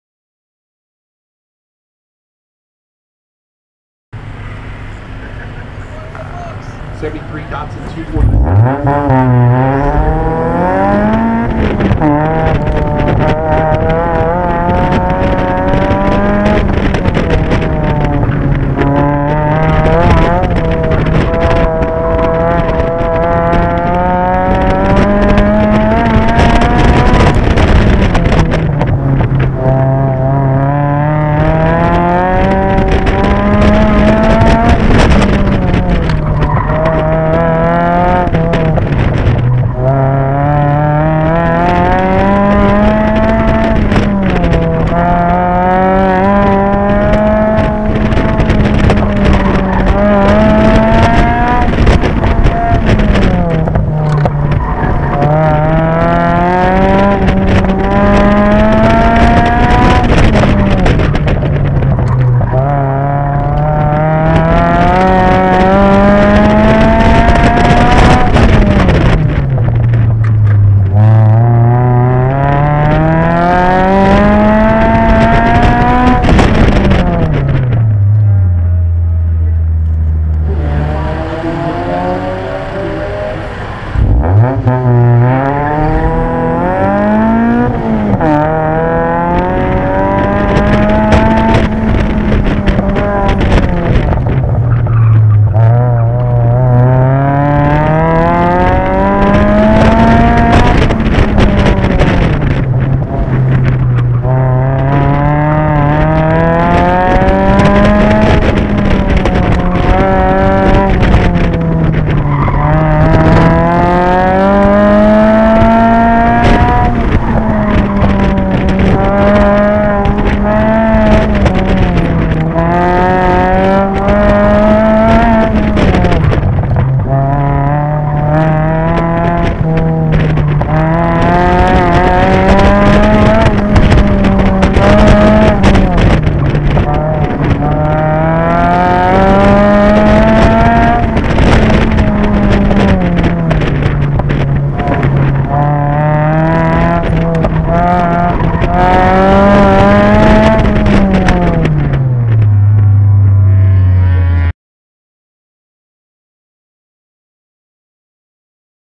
21016 SCCA DC Tour Shanes Best runs
Here are my best runs from the DC Tour.  Good for 3rd place in the Street Touring class

STR 182